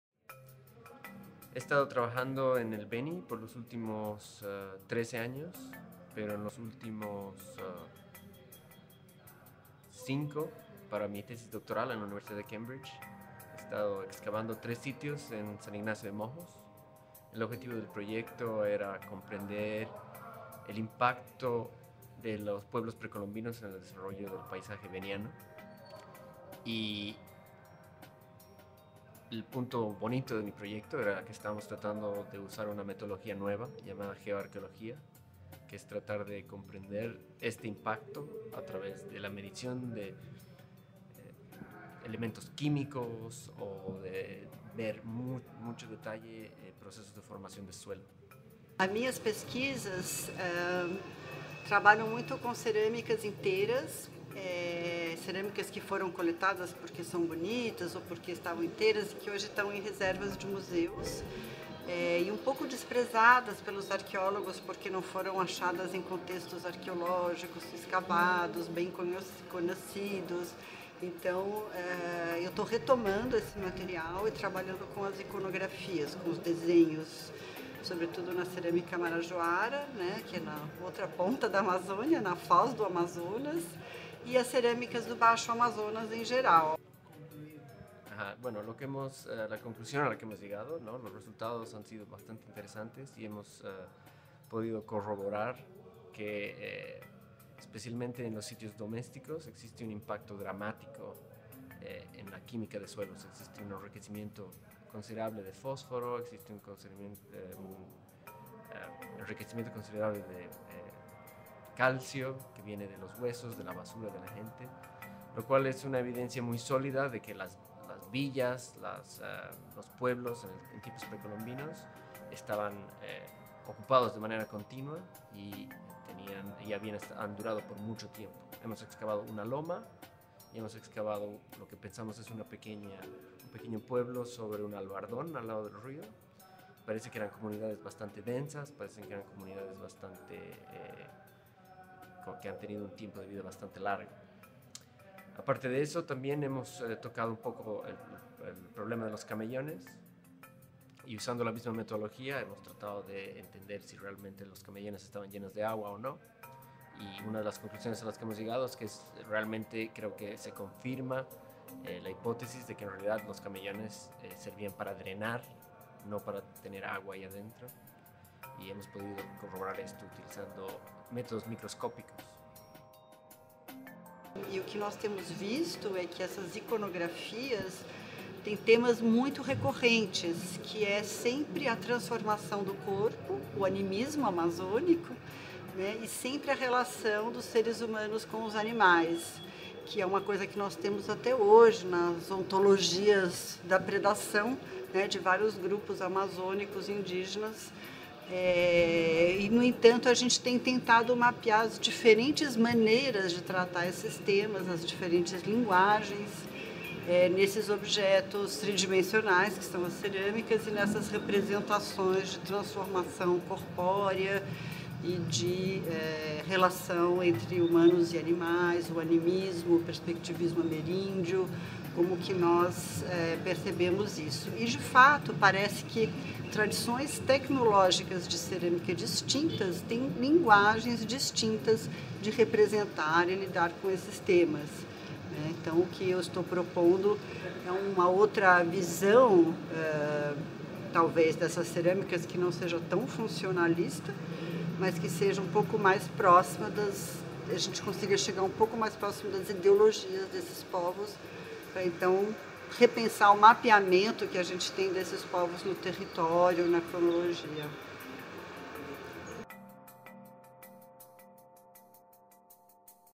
Eduardo Machicado y Cristiana Barreto: Arqueología en la Amazonía
Entrevista con Eduardo Machicado, Doctor en Arqueología por la Universidad de Cambridge, y Cristiana Barreto, Doctora en Arqueología por la Universidad de Sao Paulo.